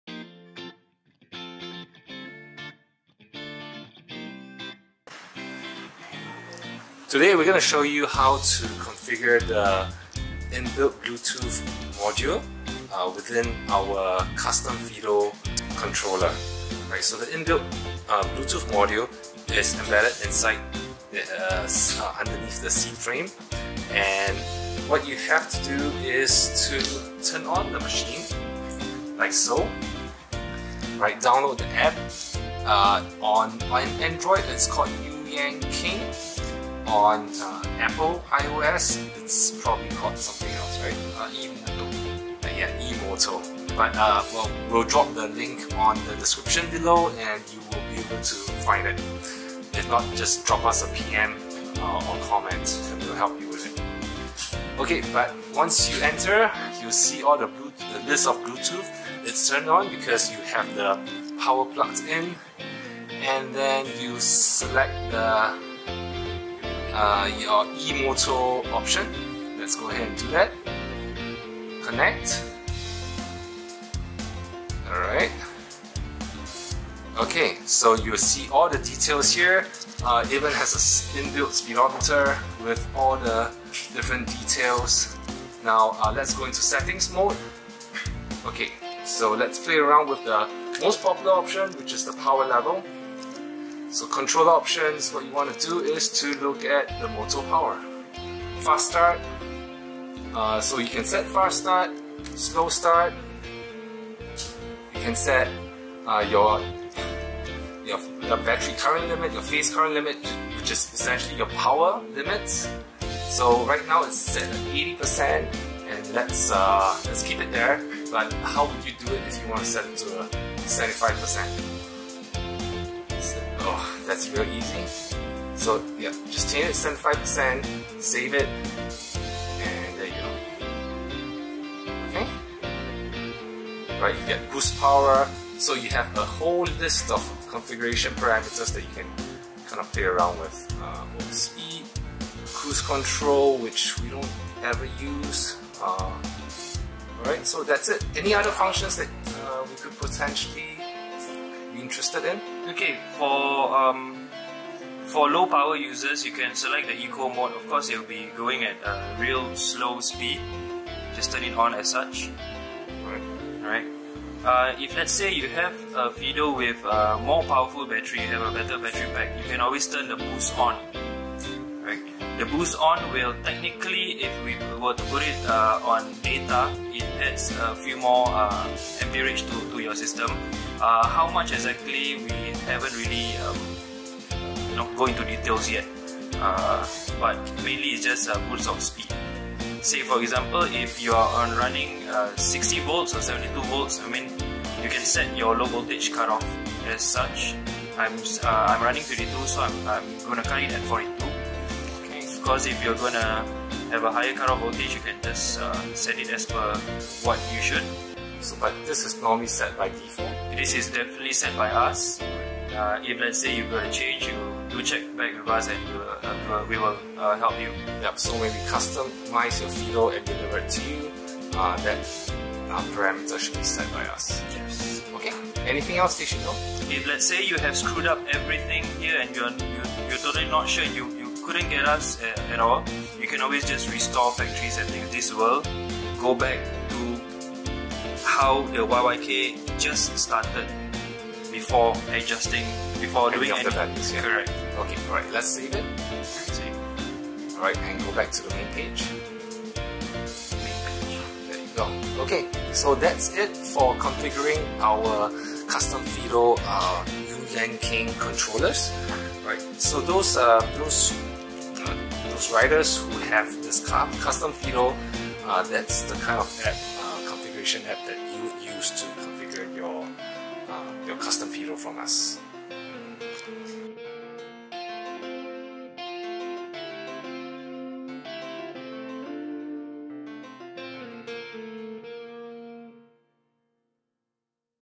Configuring Custom Fiido Squarewave Controller App
Heres how to download the YYK square wave controller app that we use on our entry level Fiido upgrades. On Android, the app is called "Yuyangking" and on iOS, the app is called "Emotor".

This YYK app is extremely easy to use and very configurable. Just be careful when adjusting each parameter. You need to test out each parameter as you adjust them and NOT adjust multiple parameters at one go!

The important parameters are usually fast/slow start to adjust startup acceleration speed, Phase or DC current level, E-brake / regen level etc.

Find out more about the featured product and more amazing e-scooter and our custom services, pls check out